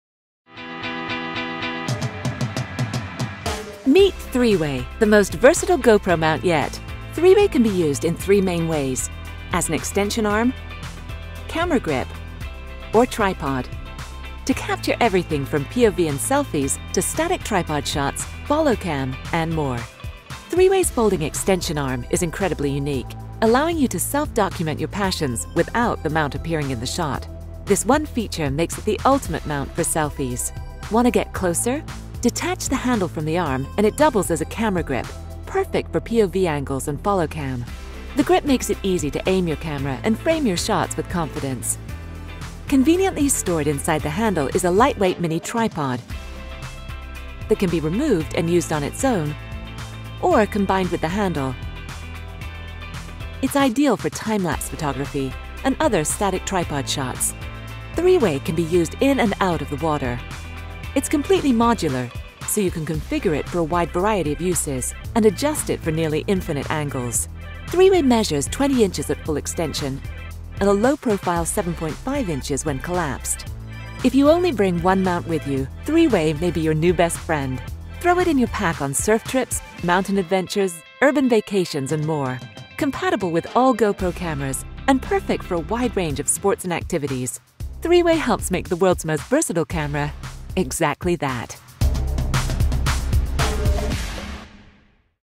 Suporte 3 em 1 - GoPro
Este suporte ultra-versátil pode ser usado de três formas diferentes: como um bastão de mão, haste de extensão ou tripé. O braço dobrável é perfeito para POV ou gravação seguida pela câmera e facilita capturar selfies sem o suporte que aparece na foto. Quando separado do braço, a alça dobra como uma garra de câmara. Armazenado dentro da alça está um mini tripé, leve, que pode ser removido e usado sozinho ou combinado com a alça. Se expande para 20" (50,8 cm), e recolhe para 7,5" (19 cm). Construção à prova d'água para uso dentro e fora da água.